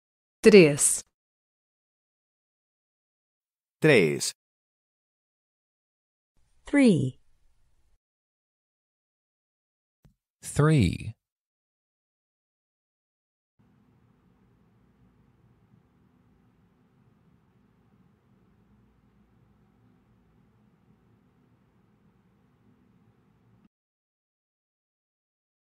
How to say "Three" in Portuguese
Male Voice:
Três

Female Voice:
Três

Male Voice Translation:
Three

Female Voice Translation:
Three